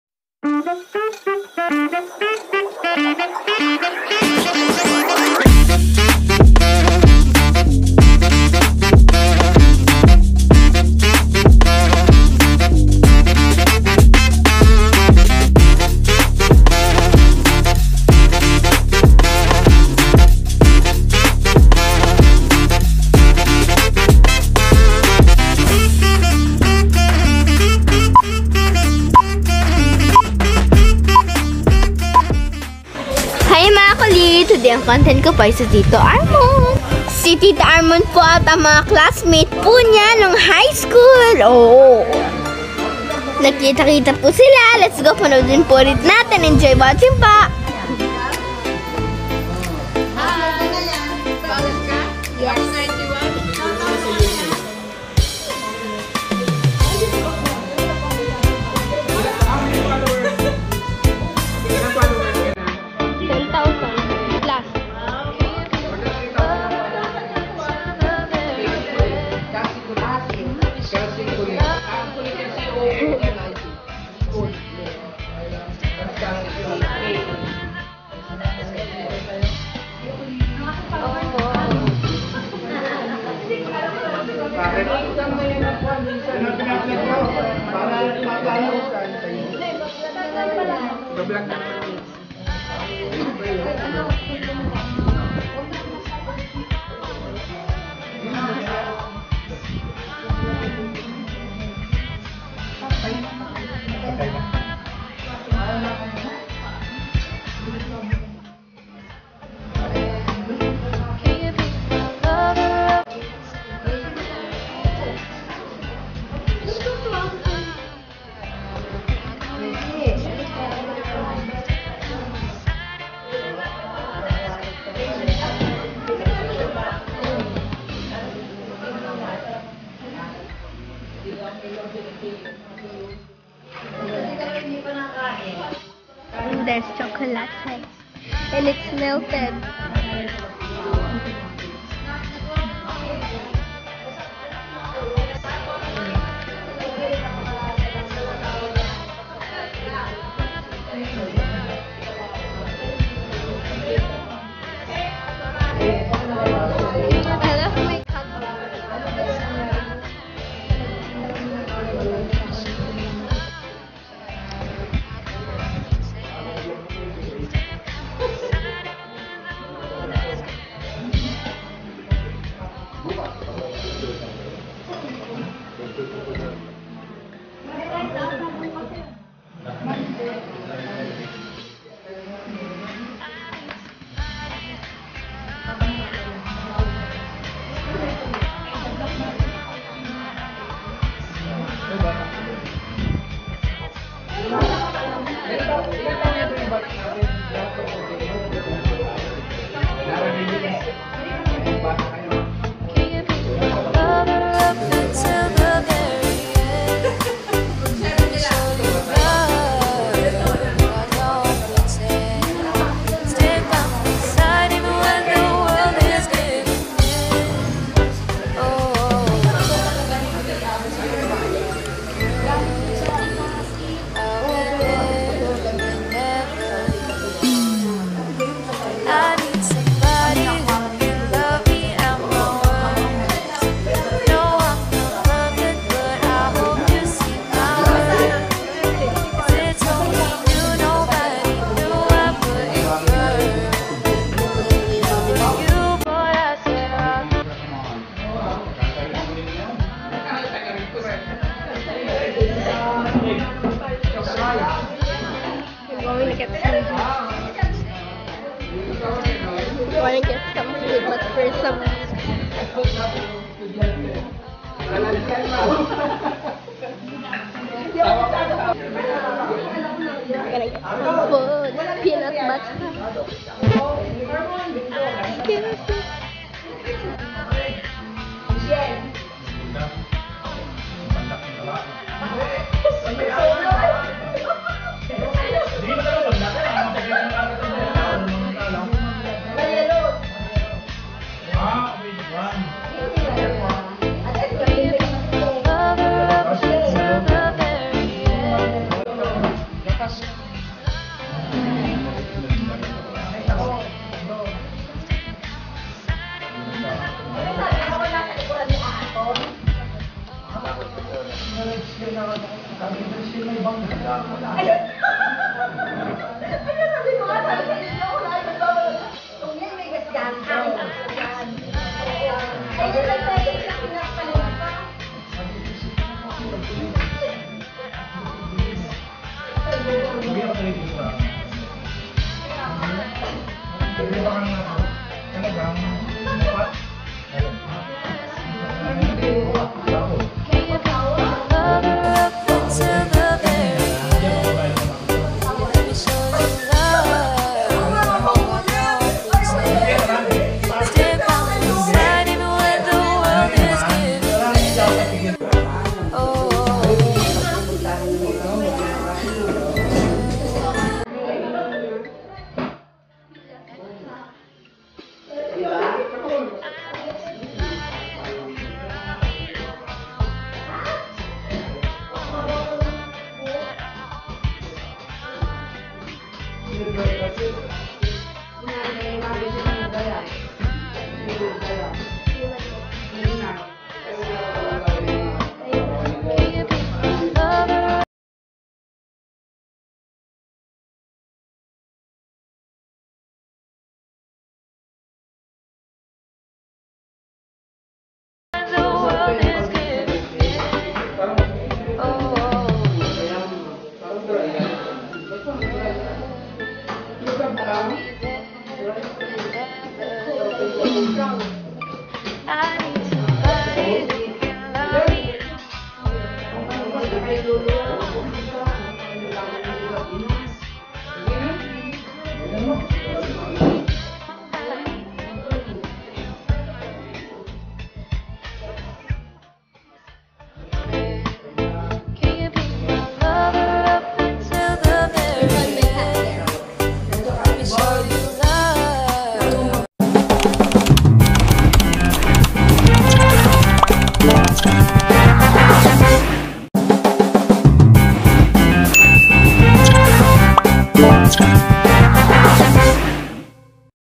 Tito Armond High School Classmates Get Together || I'm Kassy Coolit
Hello mga ka-coolit, May get together po ang mga high school classmates po nila Tito Armond, Tara makisali po tayo, makikain at maki-Marites na rin po hahaha, Let's go guys, Pls. watch po..